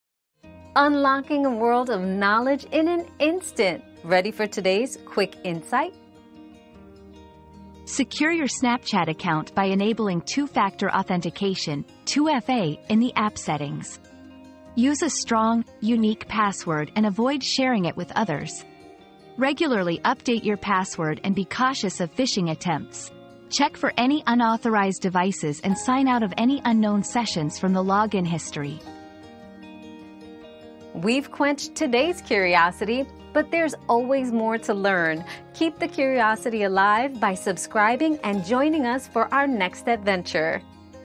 How to secure your Snapchat account from hackers?
Snapchat Security: Protect Your Account from Hackers 👉 Snapchat Security Tips 👉 Learn how to safeguard your Snapchat account from hackers by enabling Two-Factor Authentication, using a strong password, and staying vigilant against phishing scams. Keep your account secure and your personal information protected.

Laura S. Harris (2024, June 23.) How to secure your Snapchat account from hackers?
    🌐 AskAbout.video